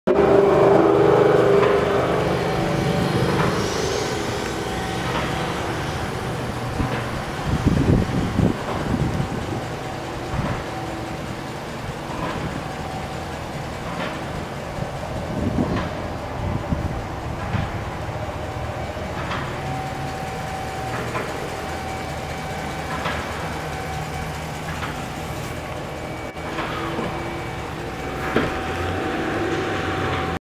2011 Dodge Ram 1500 Big Horn 1500 5.7L V8 | White | BS597633 | Seattle \ Bellevue
HEMI 5.7L V8 Multi Displacement VVT, 4WD, ** SATELLITE RADIO**, ** TOW PACKAGE**, **CLEAN AUTO CHECK**, **HEATED SEATS**, **LEATHER**, and **LOCAL TRADE**.